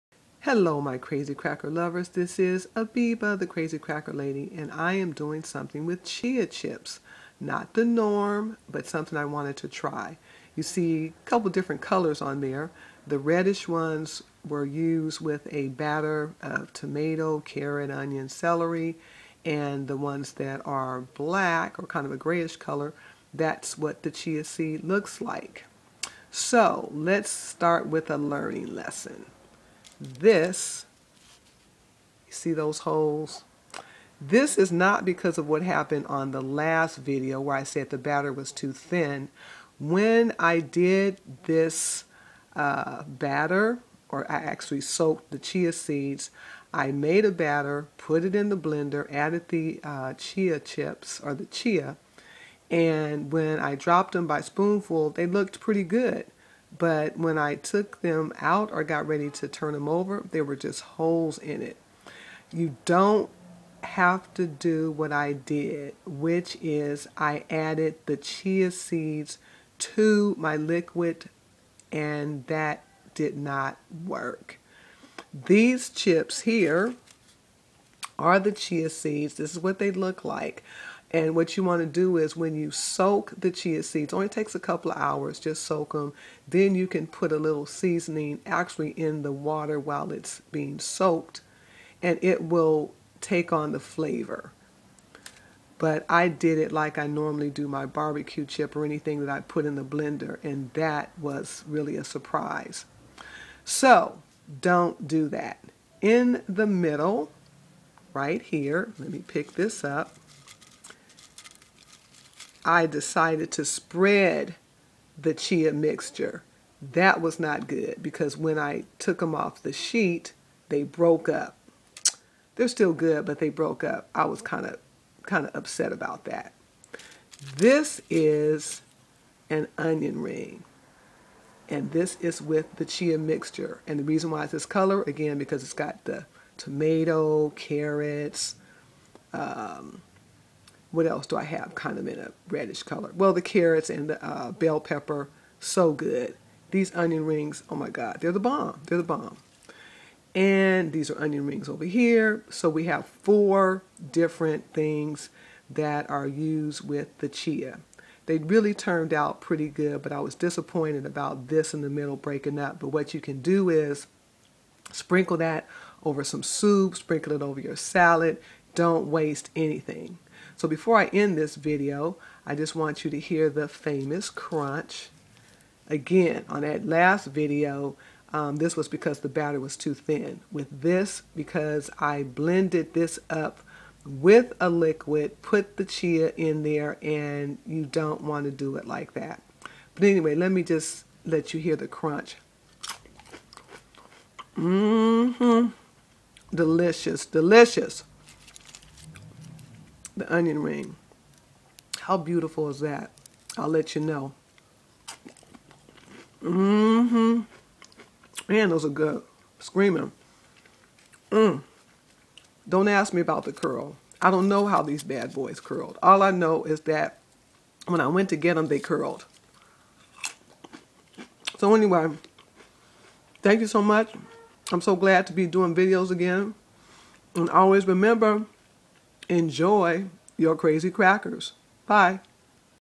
learning lesson
More learning lessons and this time with Chia seeds. Trying to do some new things with Chia krackers, let me know what you think